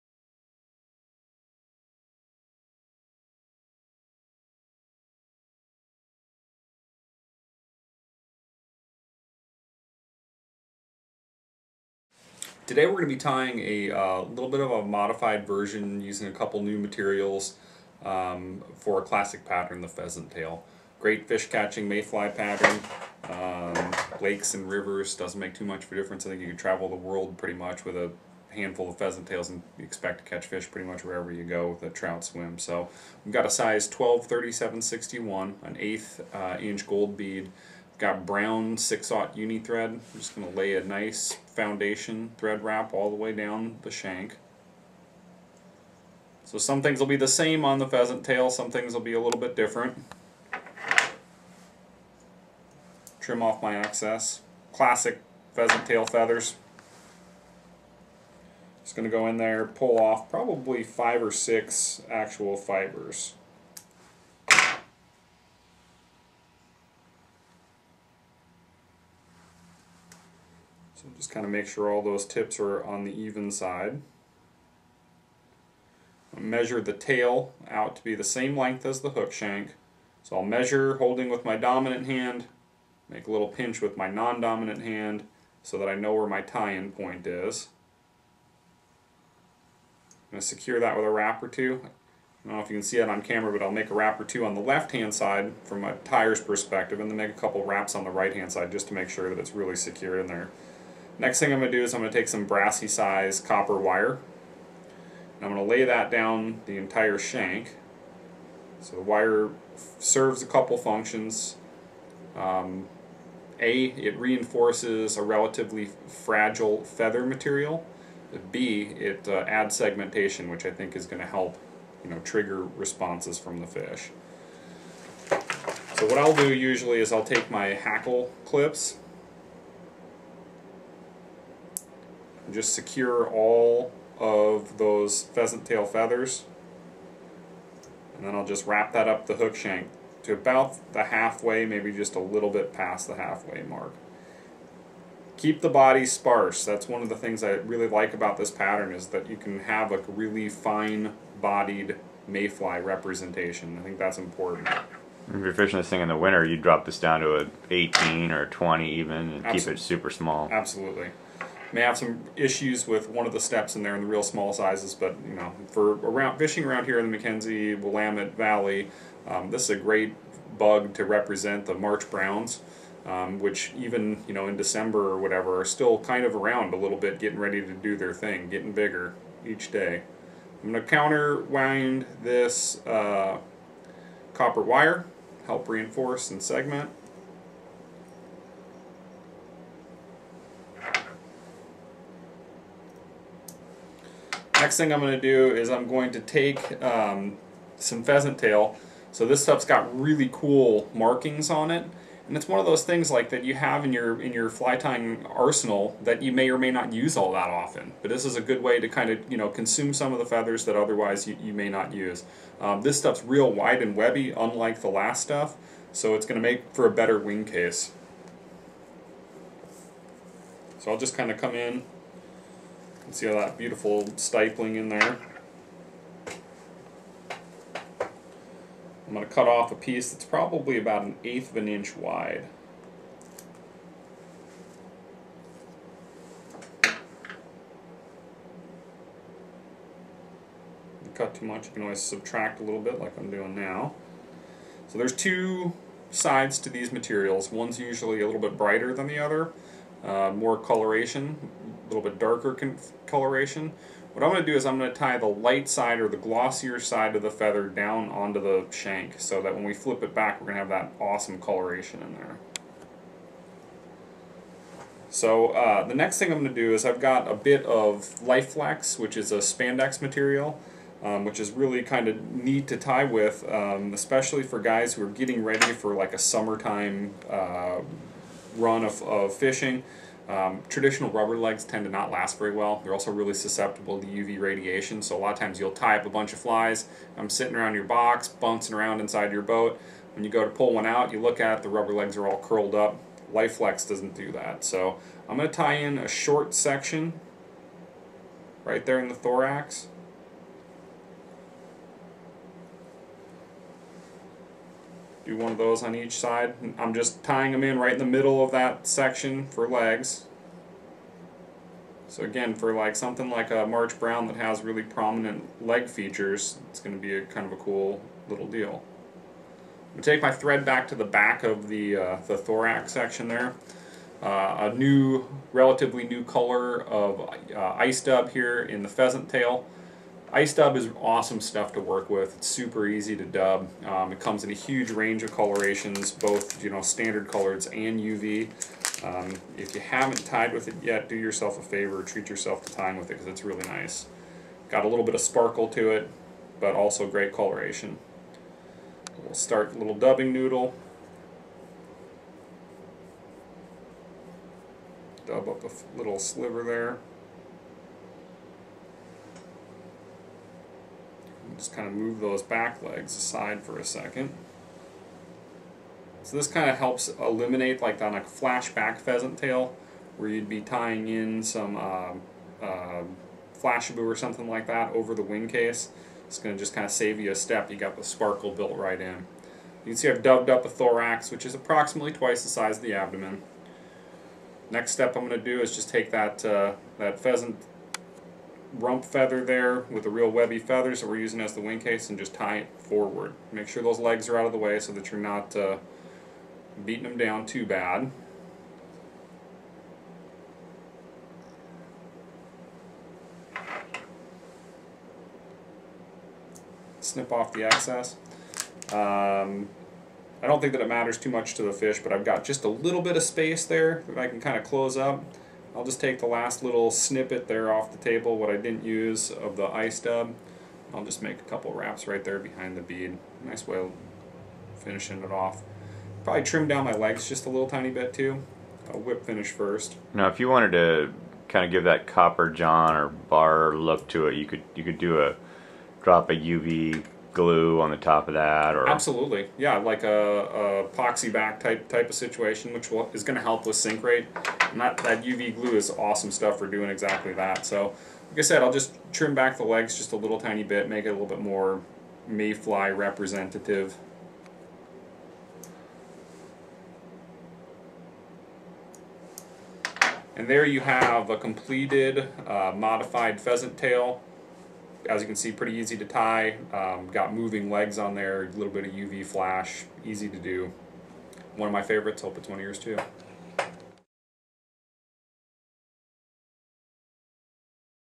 Ice Dub Pheasant Tail Nymph
The Pheasant tail nymph is one of the most effective nymph fly patterns ever created. In this fly tying video from OregonFlyFishingBlog.com Ice Dub from Hareline Dubbin is added to the thorax of the fly to help durability and flash to the fly.